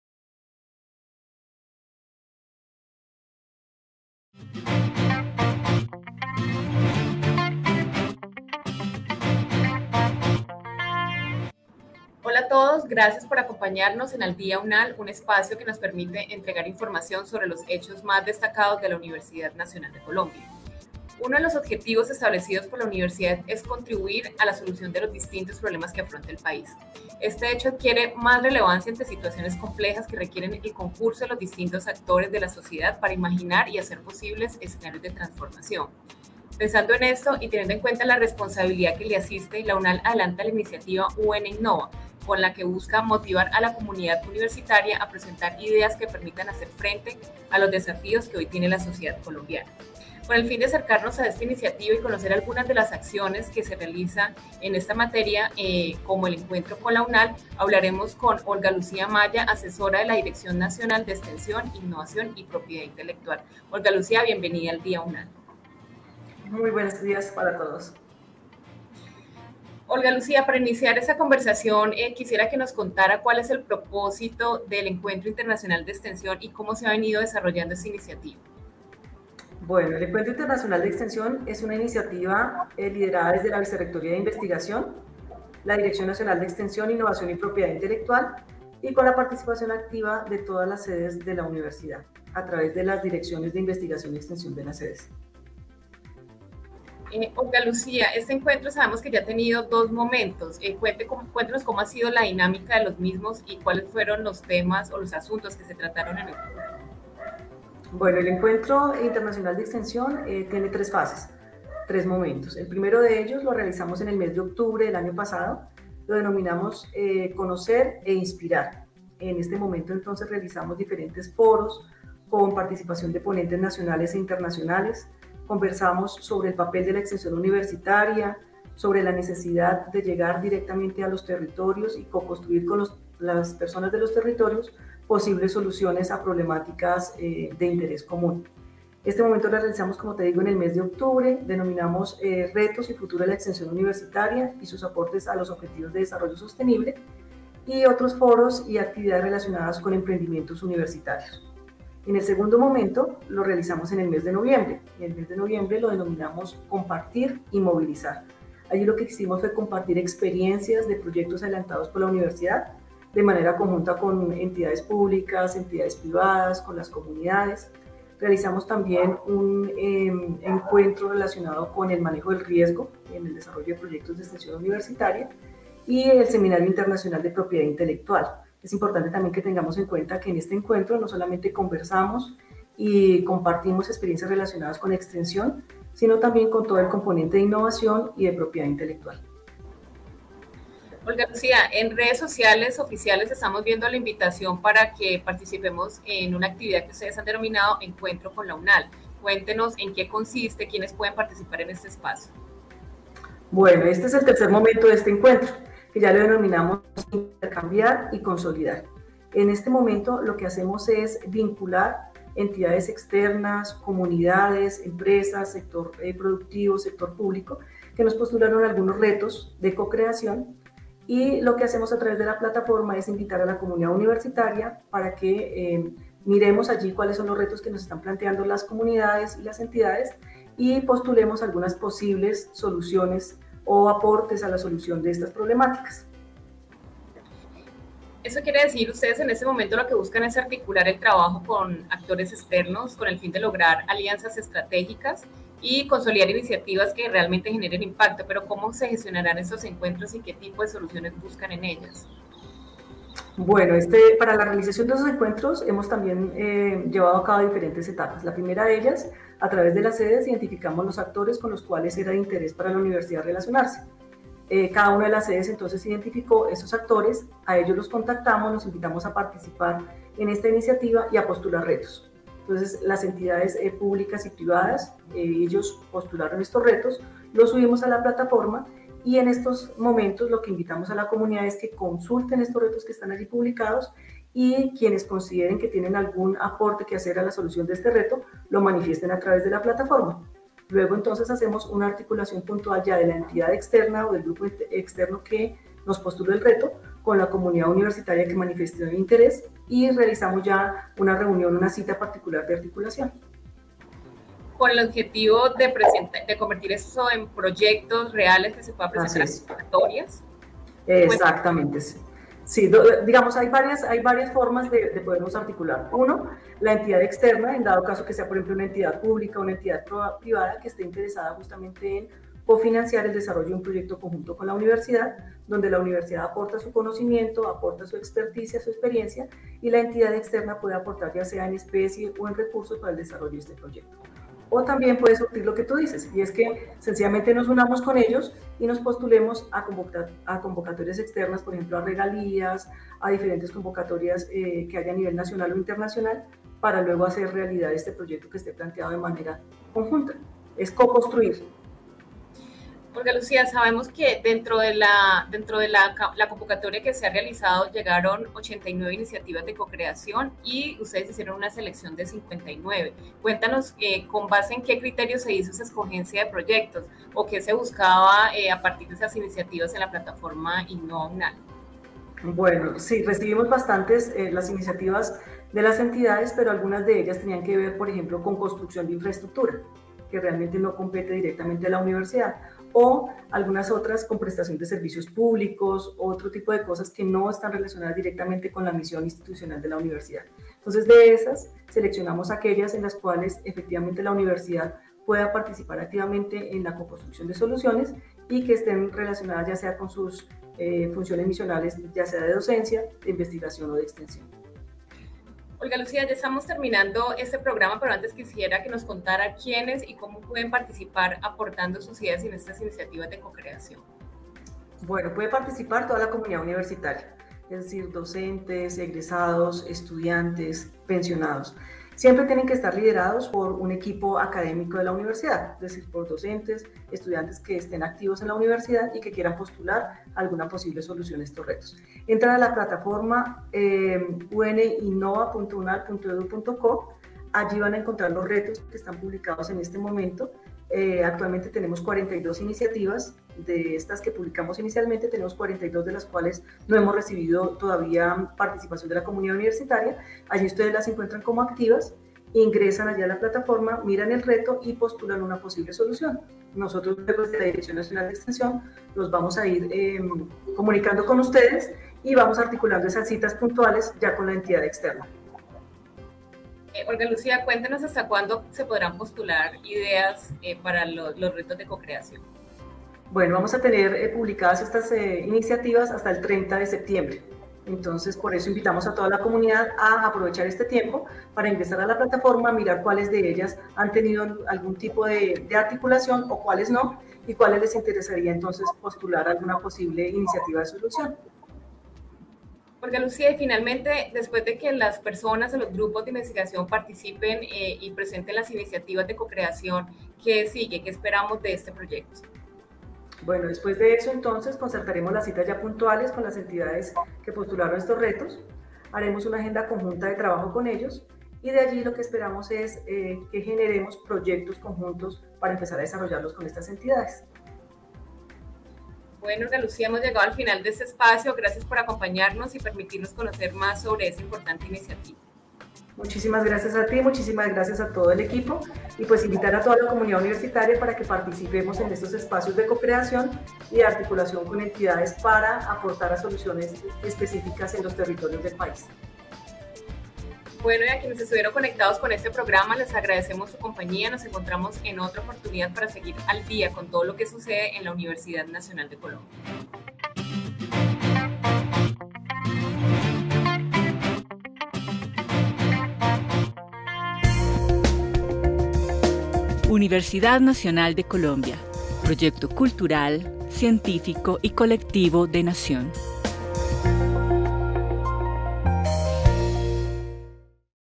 un espacio pensado para transformar la realidad del país
En esta nueva emisión del programa AlDíaUNAL hablaremos sobre el EncuentroConLaUNAL, un espacio pensado para transformar la realidad del país con Olga Lucía Amaya, asesora de UNAL